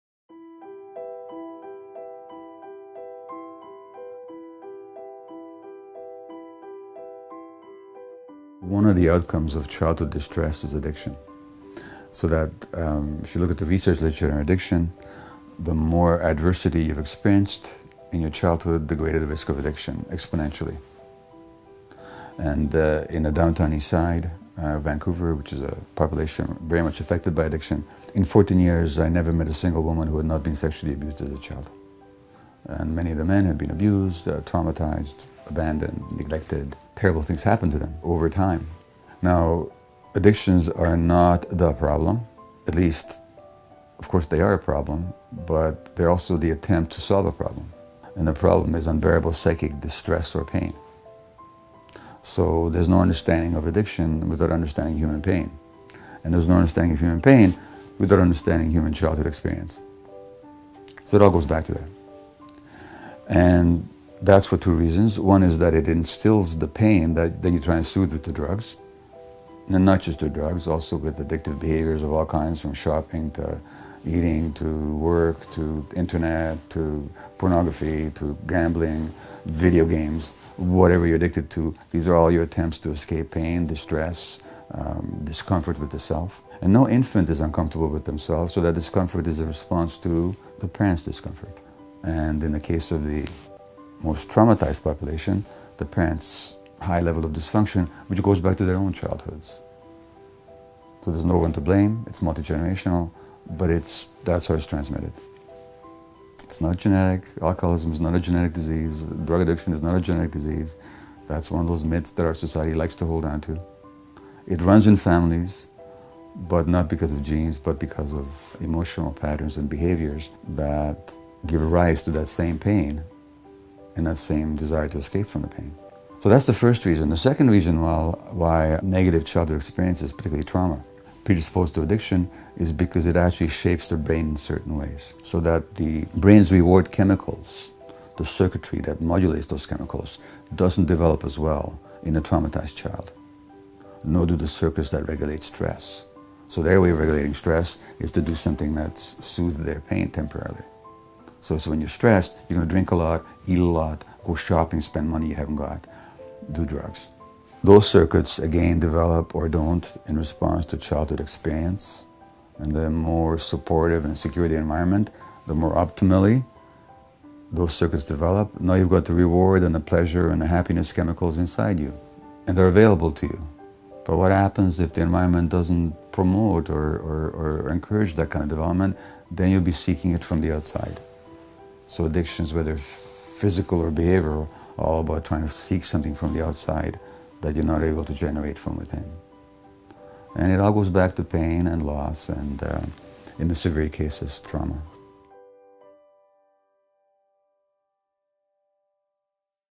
The Roots of Addiction
Painful Childhood Experiences Create a Risk of Future Addiction

Addiction is an attempt to solve the graver problem of unbearable psychic pain. To understand addiction we need to understand that human pain can come from childhood experiences. The more adversity an individual experiences in childhood the higher their risk of resorting to addictive behaviour to sooth their pain, even temporarily. Addiction (alcohol, drugs, shopping, Internet, etc.) is an attempt to seek something from the outside that the individual is not able to generate from within.

The Brain’s Ability to Deal with Stress Develops Optimally in Supportive Secure Environments

Childhood experience is significant because the circuitry that modulates the brain's reward chemicals is underdeveloped in traumatized children. Infant discomfort is a response to parental discomfort. It is also important to note that although addiction is often multigenerational and runs in families it has nothing to do with genetics but is caused by emotional patterns and behaviours that give rise to the same pain and hence, the same desire to escape from it.

Featuring:
Gabor Maté